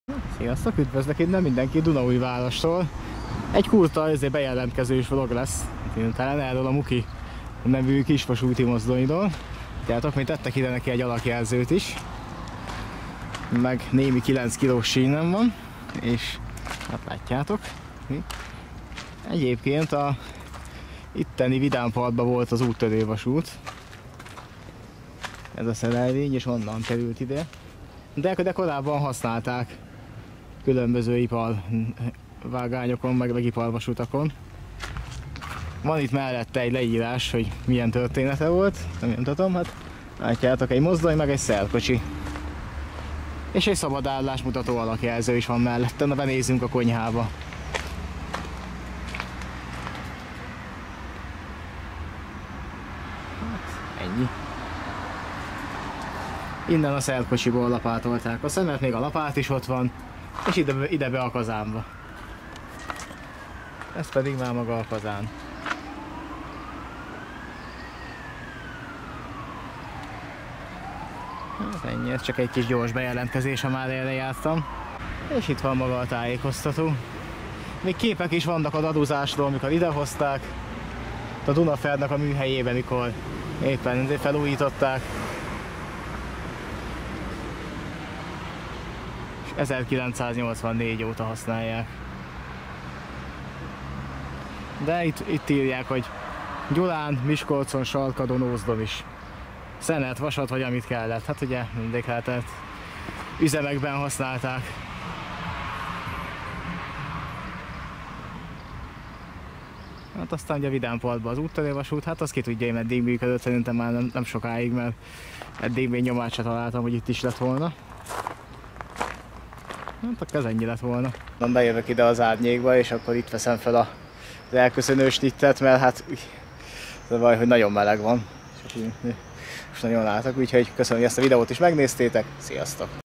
2022.07.03 Vlog Dunaújvárosi Muki mozdony
Bankszámla:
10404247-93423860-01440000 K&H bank

Muki gőzmozdony Dunaújvárosban,  ha már erre jártam akkor innen is készült egy rövid vlog.